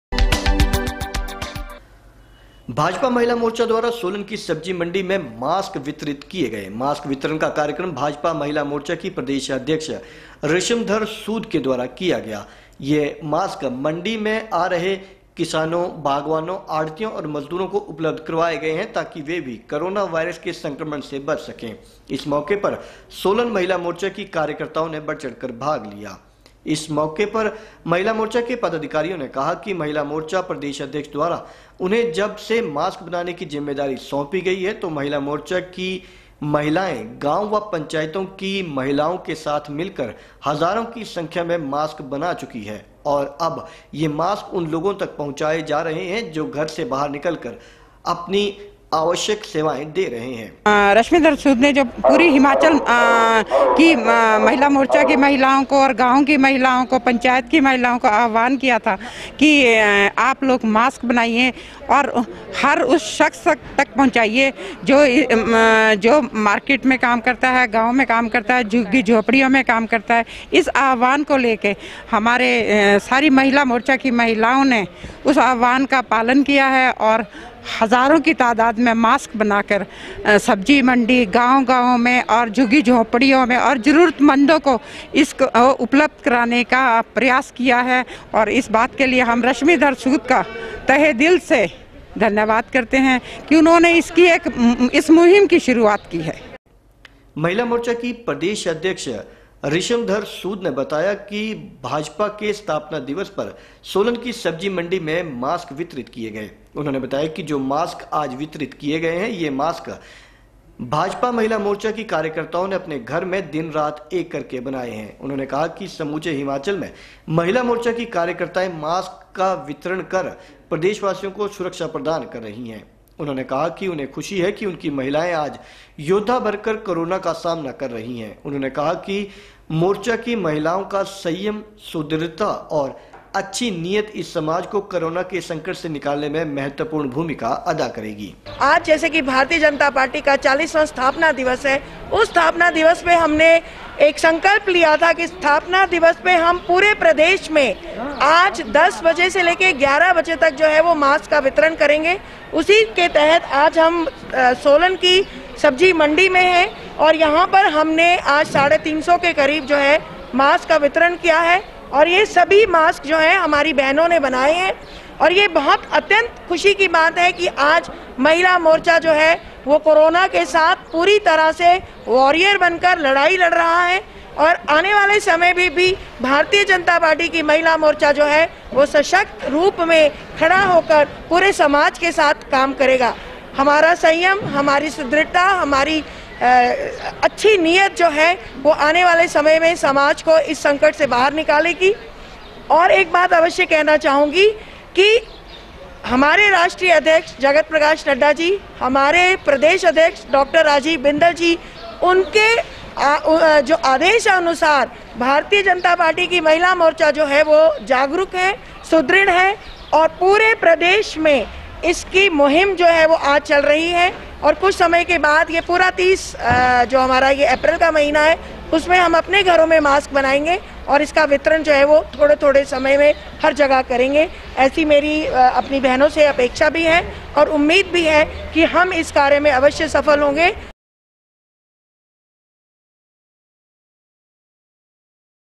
07 april 10 भाजपा के स्थापना दिवस पर महिला मोर्चा ने प्रदेश भर में बांटे मास्क ; रशिम धर सूद
भाजपा महिला मोर्चा द्वारा  सोलन की सब्जी मंडी में मास्क वितरण  किए गए |  मास्क वितरण का कार्यक्रम भाजपा महिला मोर्चा की प्रदेशाध्यक्ष रशिम धर सूद की अध्यक्षता में आयोजित किया गया | यह मास्क मंडी में आ रहे किसानों बागबानों आढतियों और मजदूरों को उपलब्ध करवाए गए ताकि वह भी कोरोना वायसर के संक्रमन से बच सके |  इस मौके पर सोलन महिला मोर्चा की कार्यकर्ताओं ने बढ़ चढ़ कर भाग लिया |इस मौके पर महिला मोर्चा की पदाधिकारियों ने कहा कि  महिला मोर्चा  प्रदेशाध्यक्ष द्वारा उन्हें जब से मास्क बनाने की जिम्मेवारी सौंपी थी तो   मोर्चा की महिलाओं  गाँव और   पंचायतों की महिलाओं ने   हजारों की संख्या में मास्क बनाए